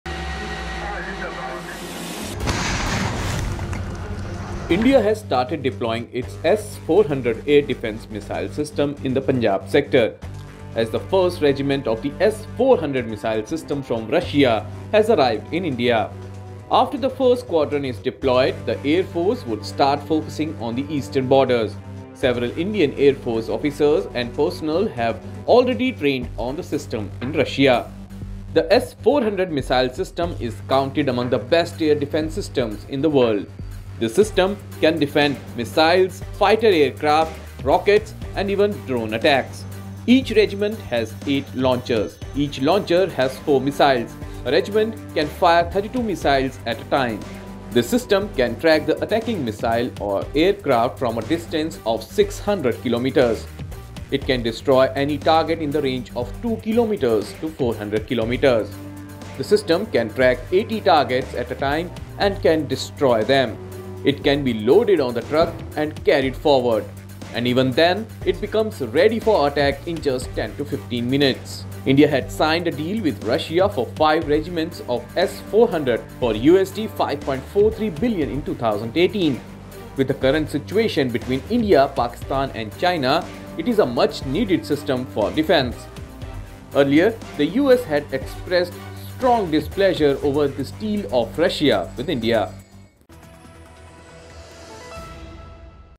Why India deployed S-400 air defence system in Punjab sector?
In a major boost to India's capability to tackle aerial threats from both China and Pakistan, the Indian Air Force (IAF) has started deploying its S-400 air defence missile system in the Punjab sector. Do you know, why is IAF deploying S-400 in Punjab Sector?